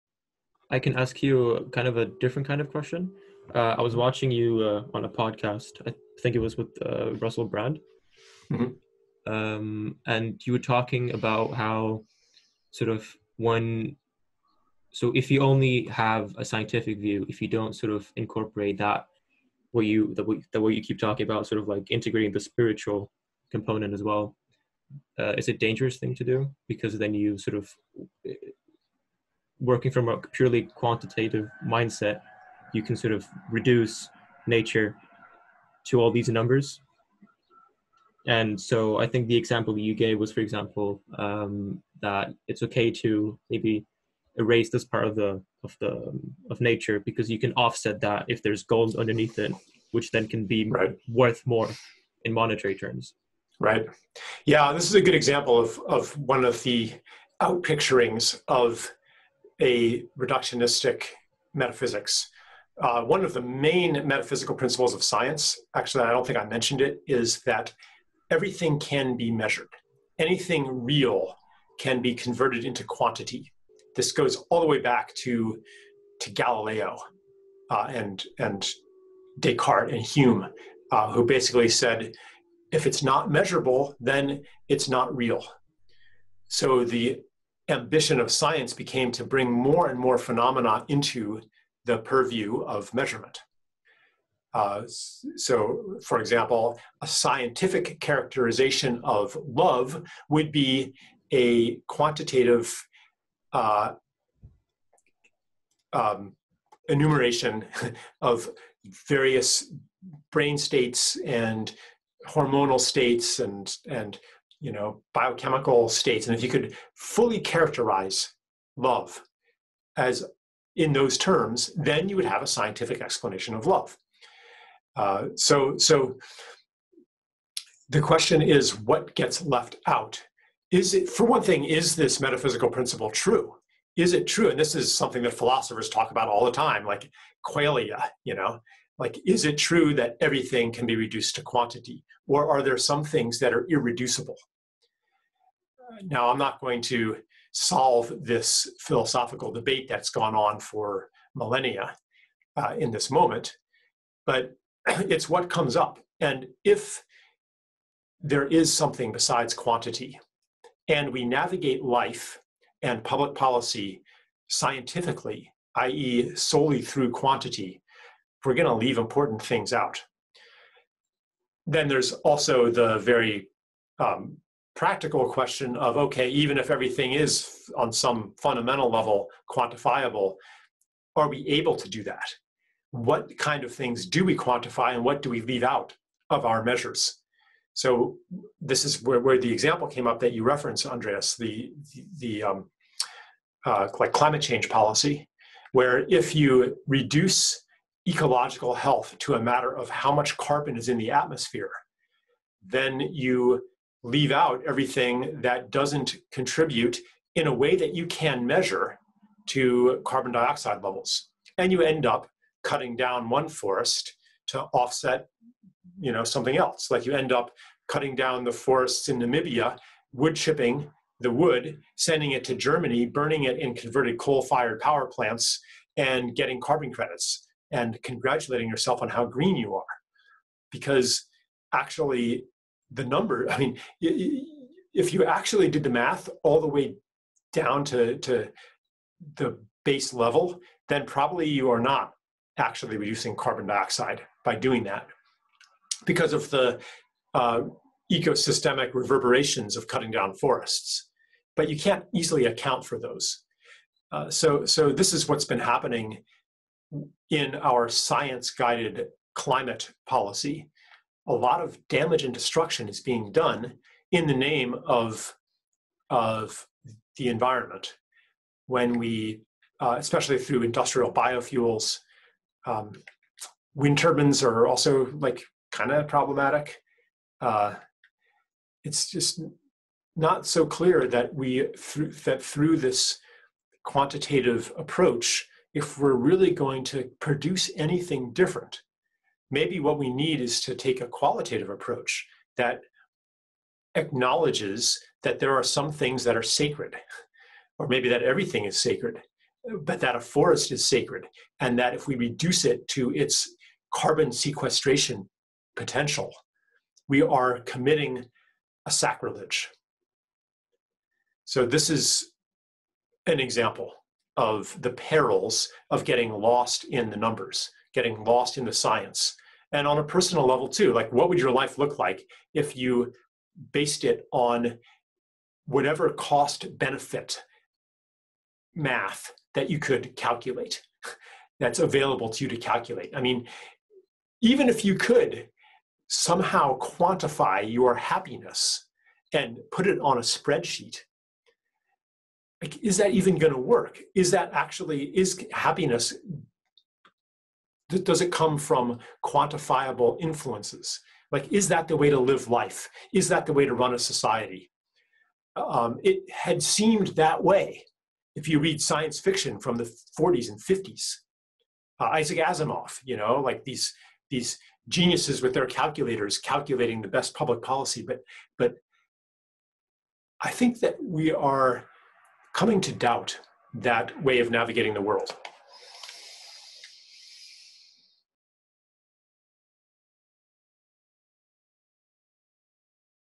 The Perils of getting Lost in Science
One of the main assumptions in science is that everything real is measurable. But when one then assumes that whatever they happen to measure or choose to measure is the only reality, things become much more Reductionist and much less useful in serving Beauty.